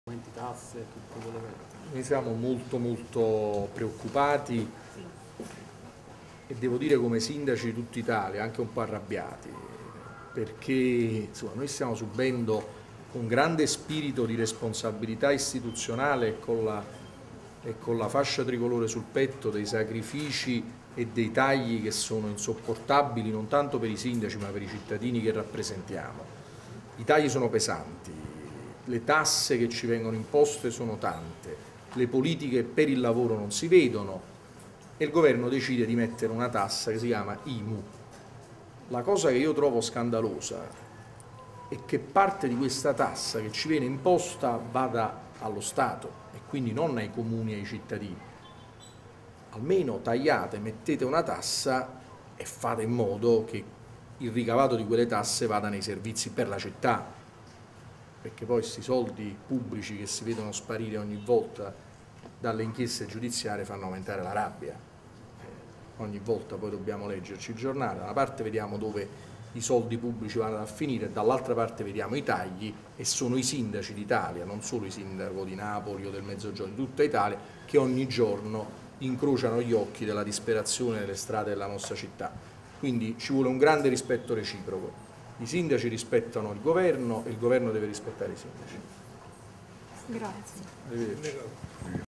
Imu: de Magistris, i sindaci rispettano il Governo ma questo rispetti i sindaci
Napoli, Palazo San Giacomo. 3 maggio 2012